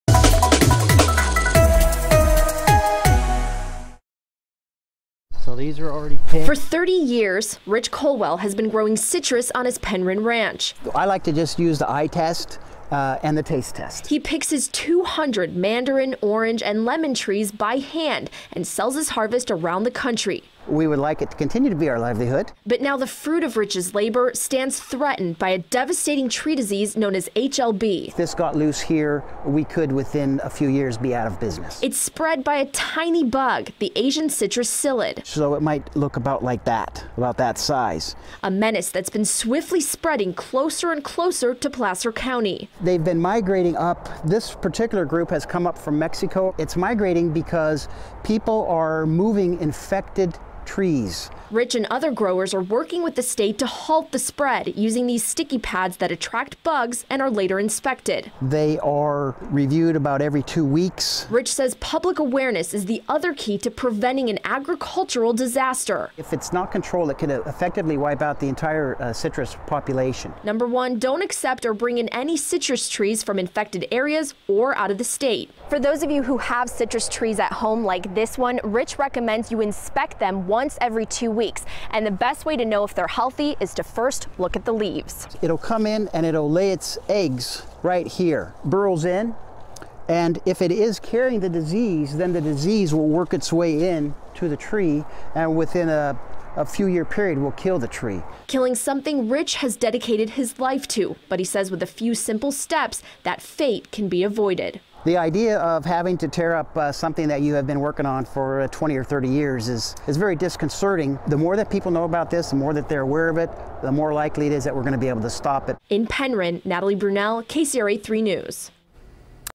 Tiny bug could kill hundreds of citrus trees in Placer County
A Placer County citrus grower said a tiny bug carrying a deadly disease could kill his citrus trees and other growers' crops.